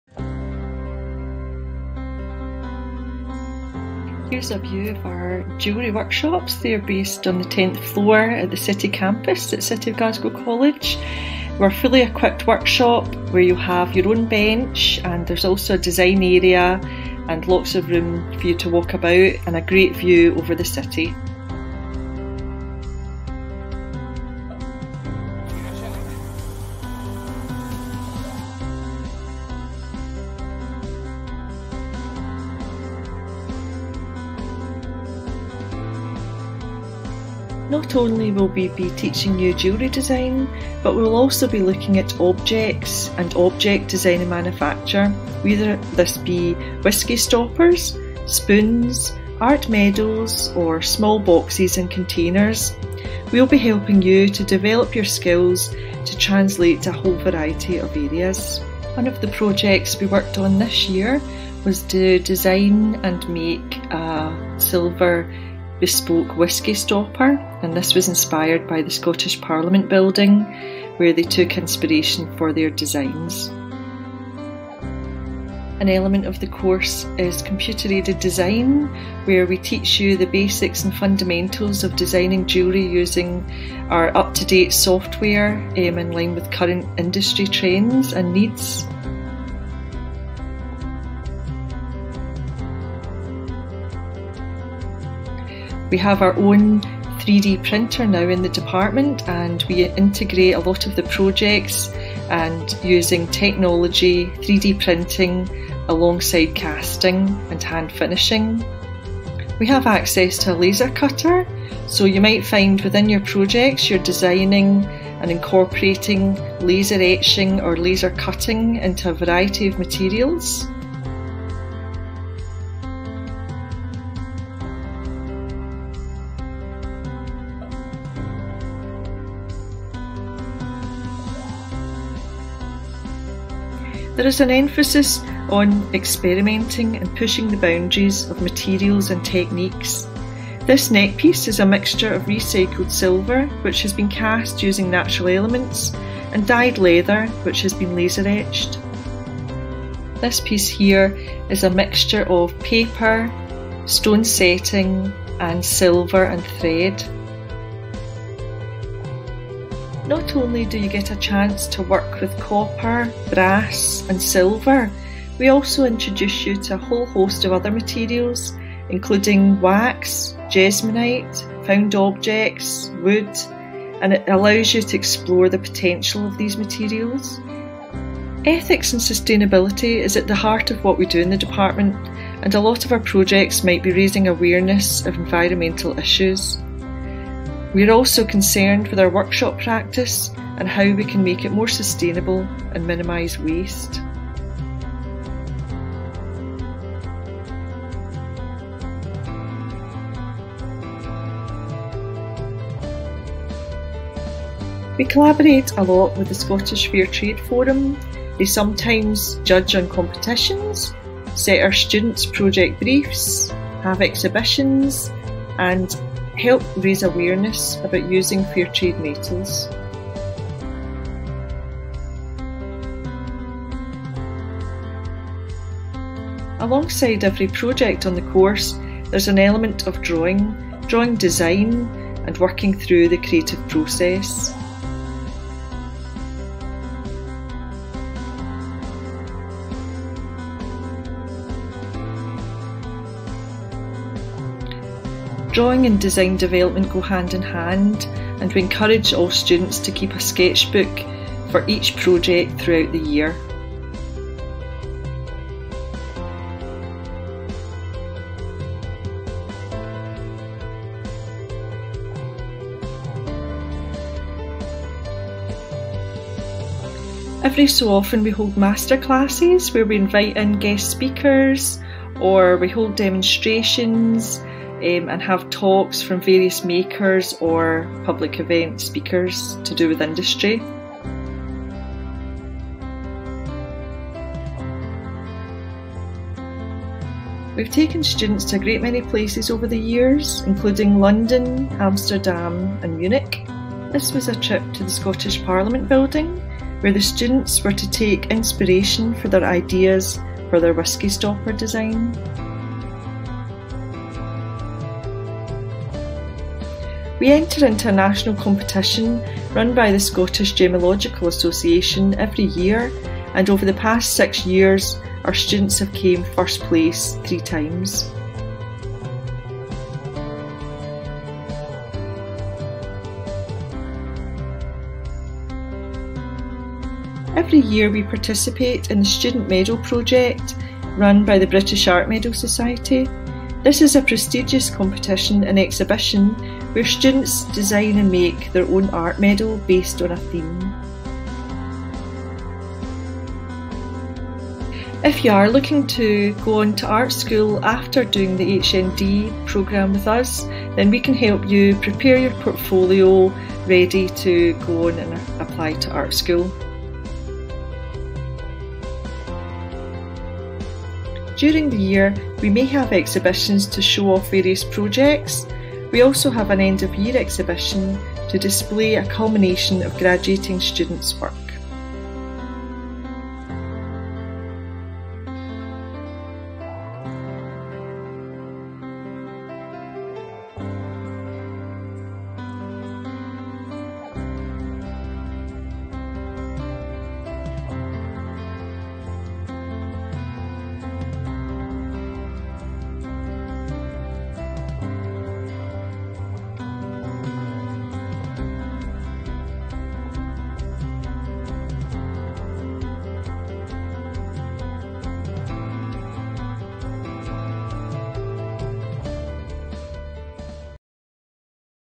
Jewellery Schools Open Day Video
Information about NQ and HND Jewellery courses at City of Glasgow College.